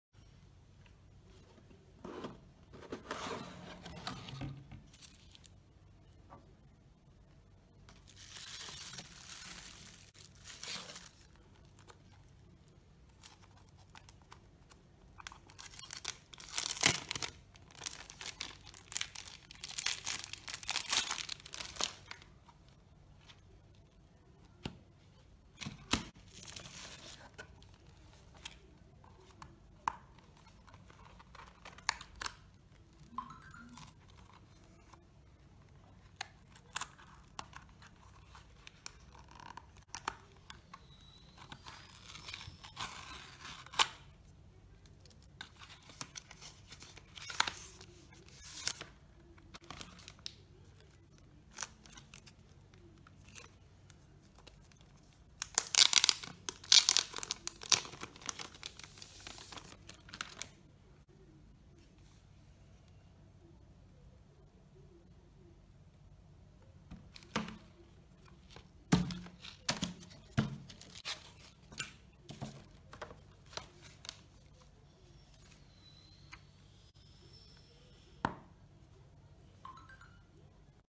Kindle Paperwhite 11th Generation - Unboxing ASMR
Assalamualaikum WR WB. A silent ASMR unboxing of the kindle paperwhite.